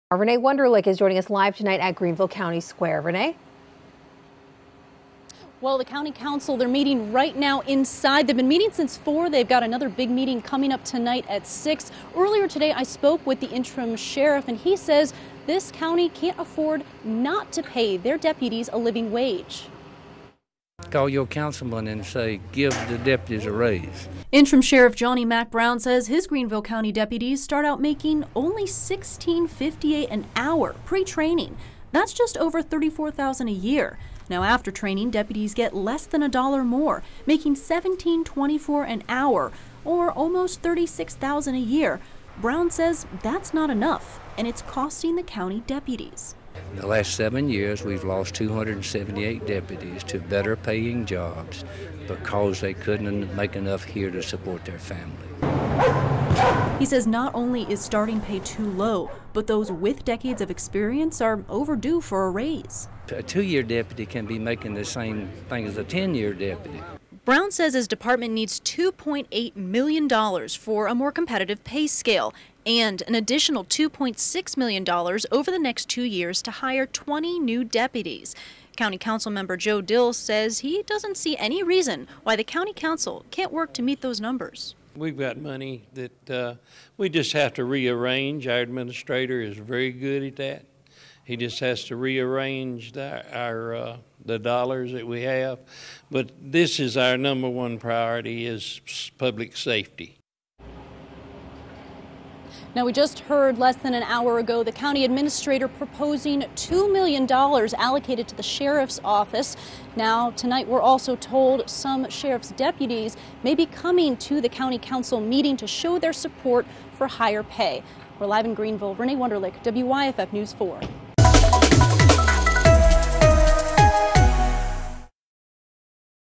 'Give the deputies a raise!' Upstate sheriff says
Greenville County Interim Sheriff Johnny Mack Brown says he's losing deputies because they can't afford to support their families